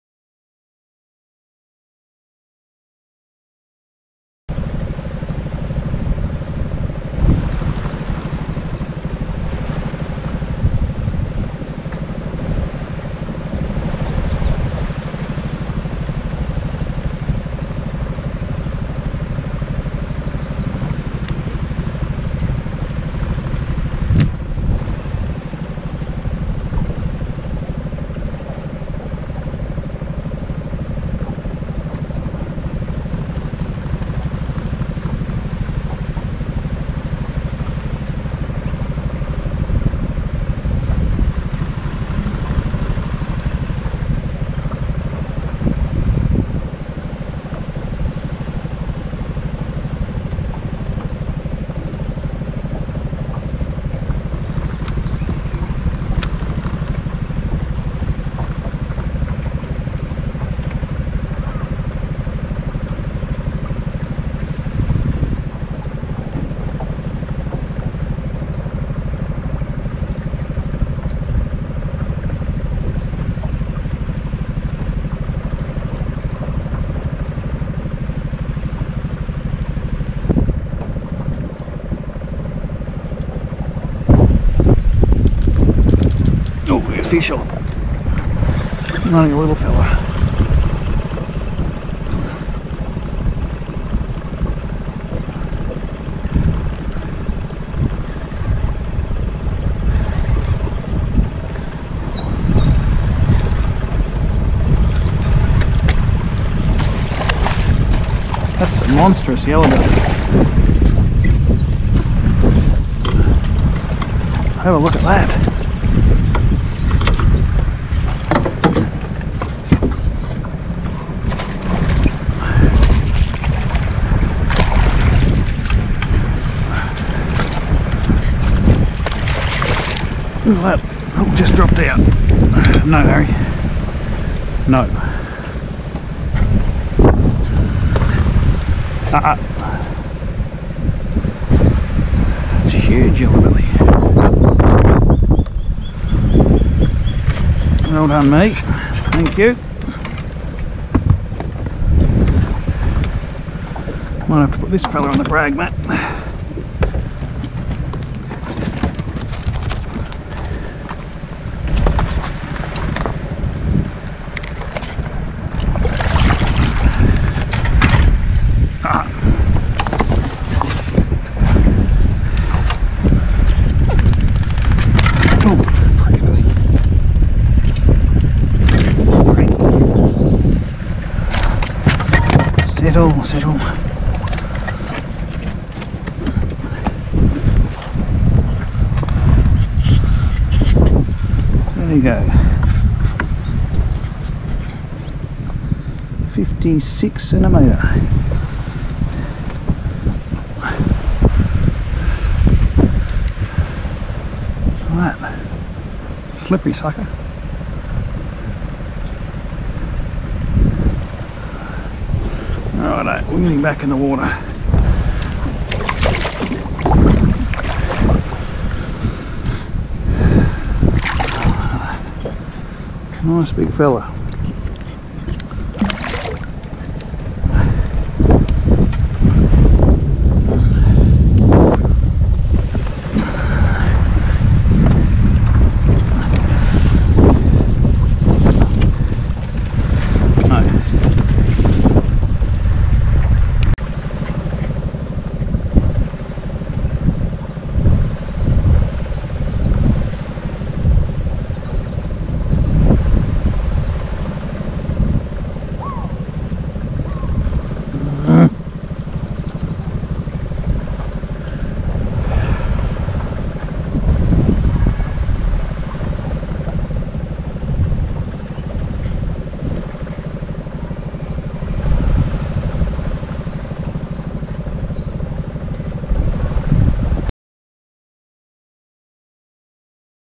Yellowbelly 56cm, Edward River Deniliquin
Snuck out for a quick troll in the centre of town resulting in a solid 56cm Yellowbelly, missed a couple as well